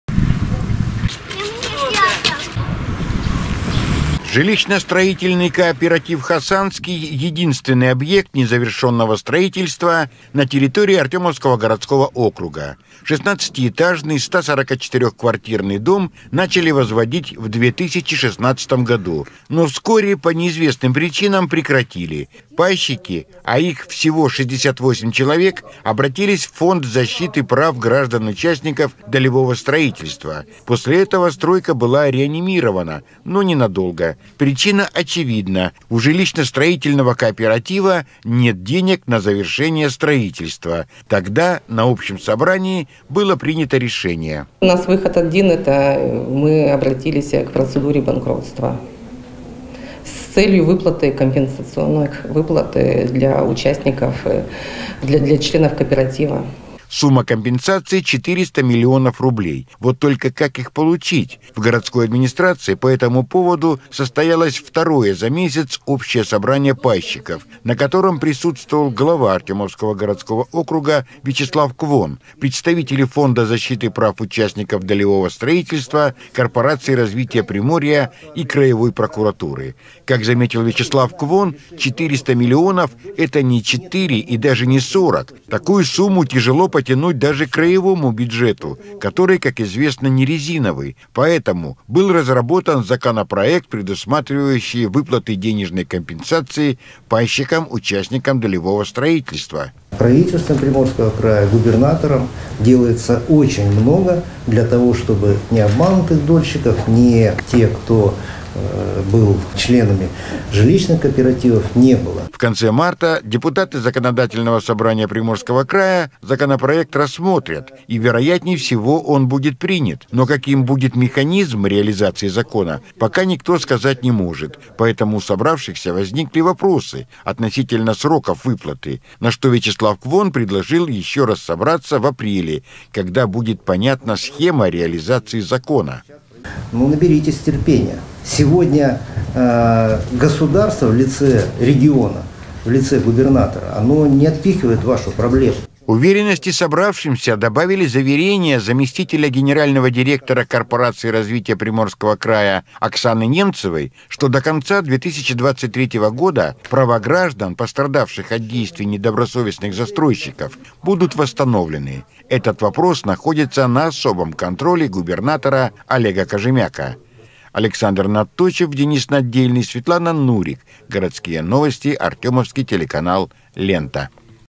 Под контролем губернатора. Проблема долгостроя ЖСК «Хасанкий» должна быть решена
В Приморье на решение проблемы долгостроя ЖСК «Хасанский» планируется выделить порядка 400 миллионов рублей. В городской администрации состоялась вторая в этом году встреча с пайщиками ЖСК «Хасанский», под руководством регионального Фонда защиты прав участников долевого строительства, Корпорации развития Приморского края, с участием краевой прокуратуры и главы АГО, Вячеслава Квона